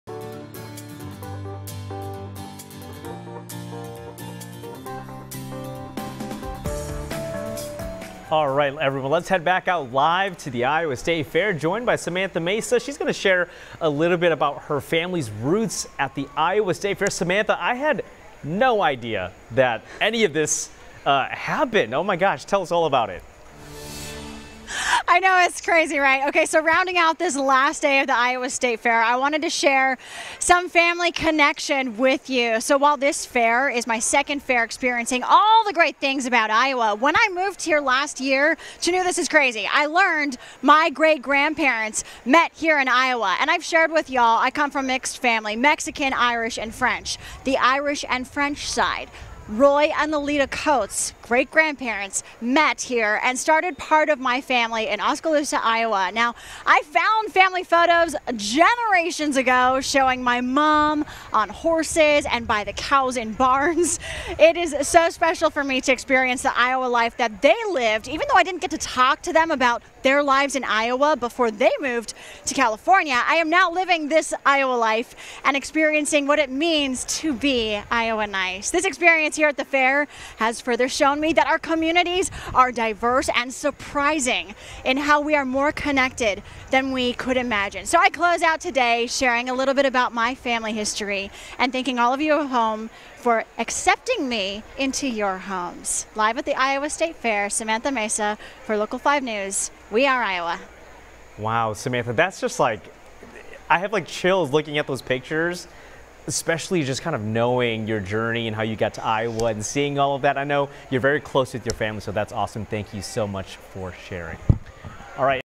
Local 5 anchor Samantha Mesa shares her family ties to the Iowa State Fair
While 2022 is only the second year Mesa has attended the fair, her family history with the fair goes way back: her great-grandparents actually met at the fair.